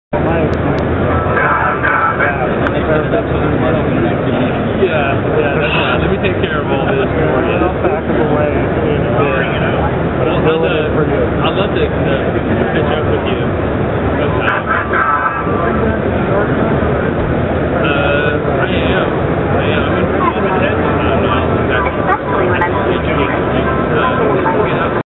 Digital Signage Expo: Connected Coke Machine
Rsupport Presents:  Some of the new and interesting technology seen at the DSE 2011.